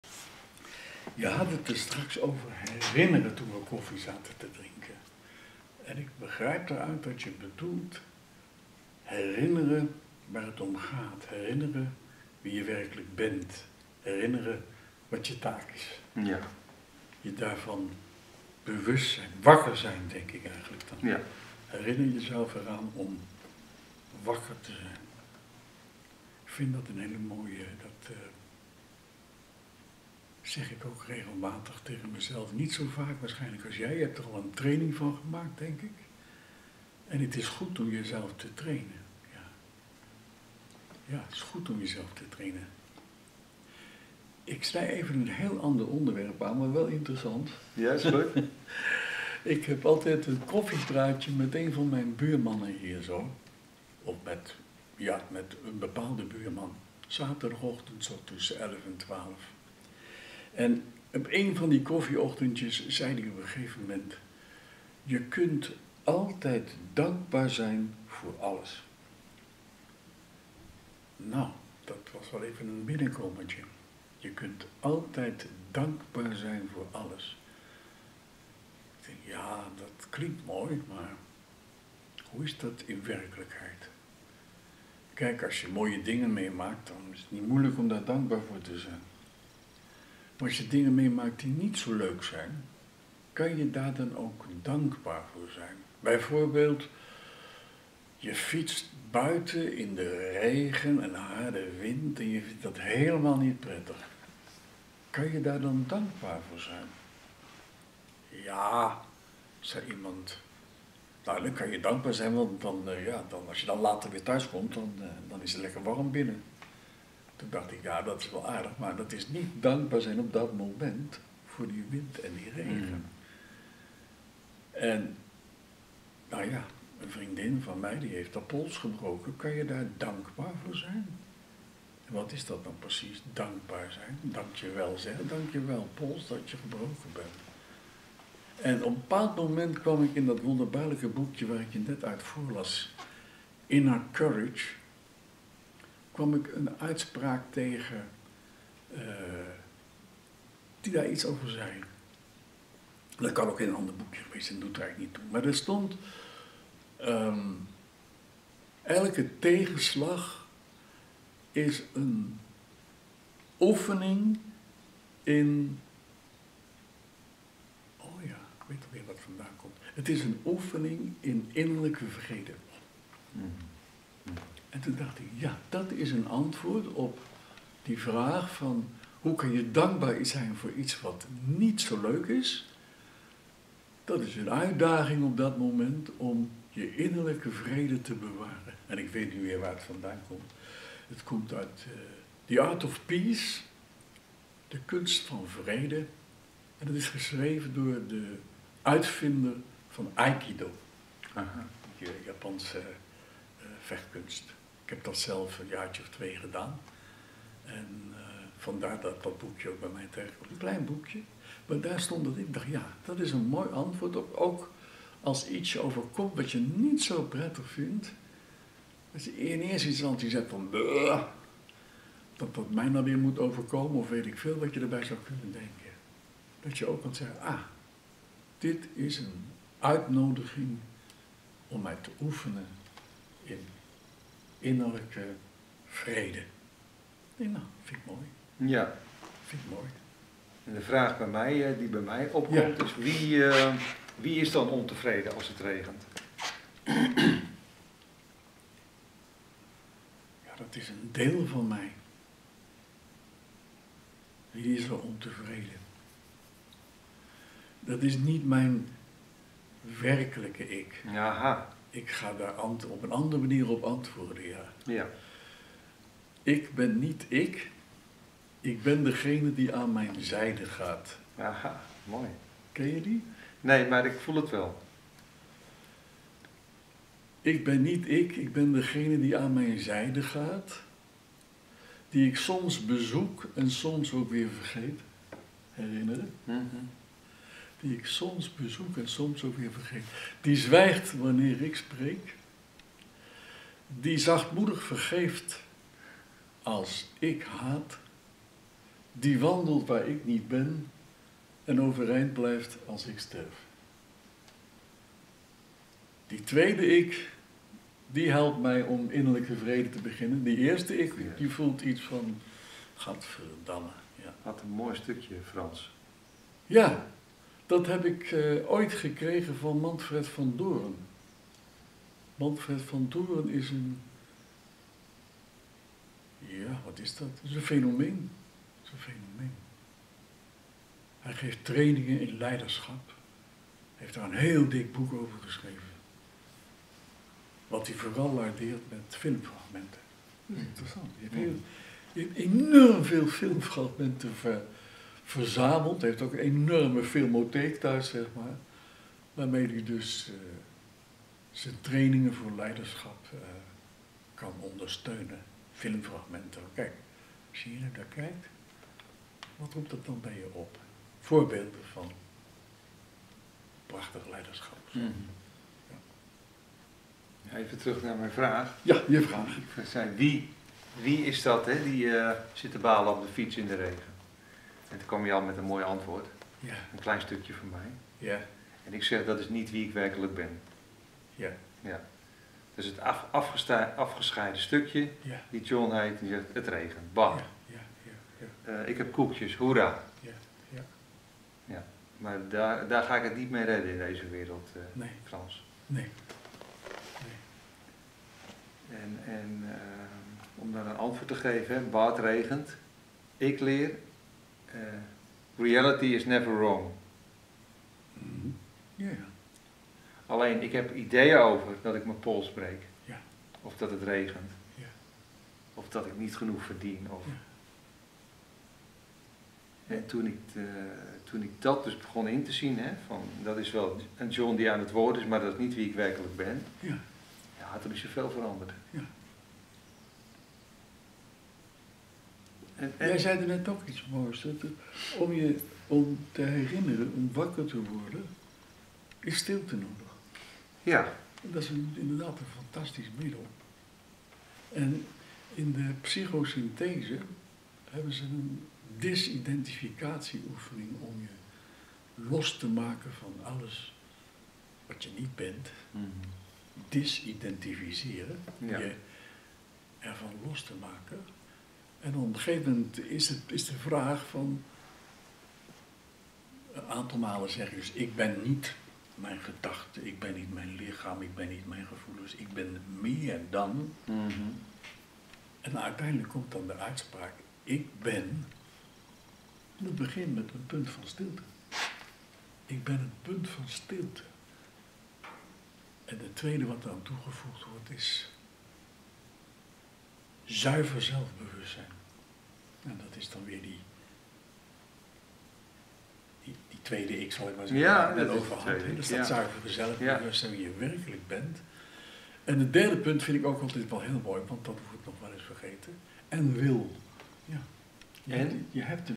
HERINNERING. Herinneren aan wie ik werkelijk ben. Mijmeren met mijn vriend Frans de Vette.
Filosofie, gezonde zelf liefde, vriendschap.
Non dualiteit.